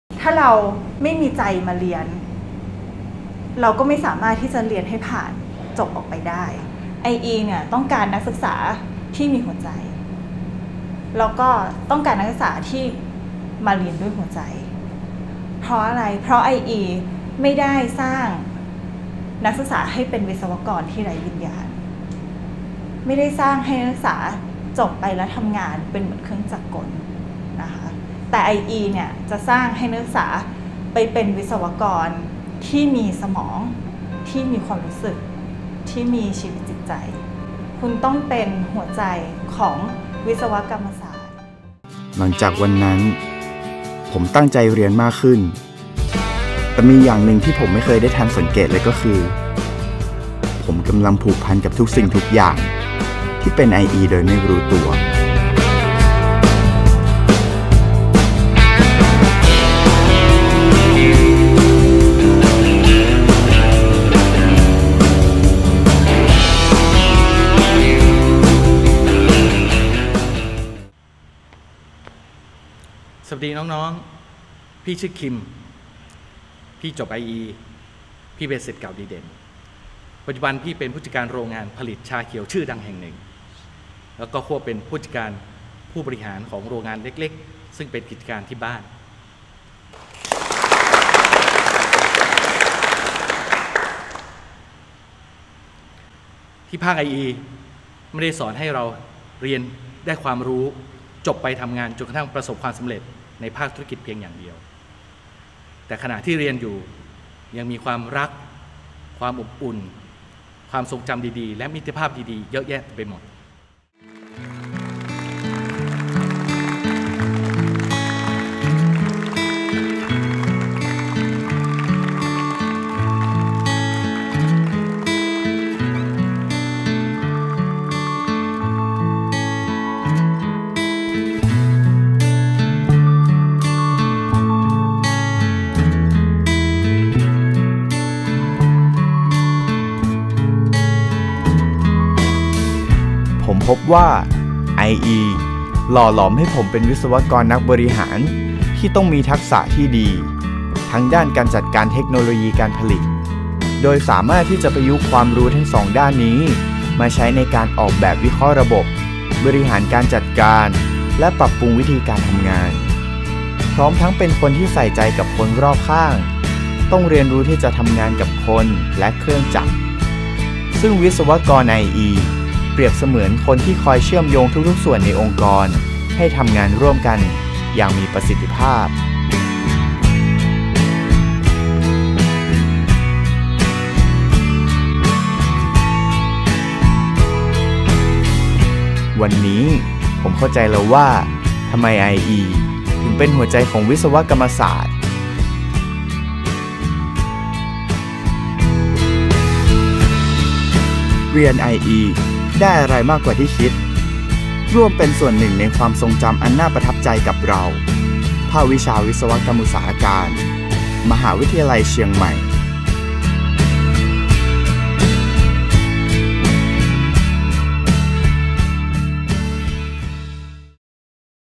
เรียน ไออี มช. ได้อะไรมากกว่าที่คิด
แนะนำภาควิชาวิศวกรรมอุตสาหการ
คณะวิศวกรรมศาสตร์ มหาวิทยาลัยเชียงใหม่

ติดตามภาพยนตร์สั้น (ฉบับเต็ม) ได้ที่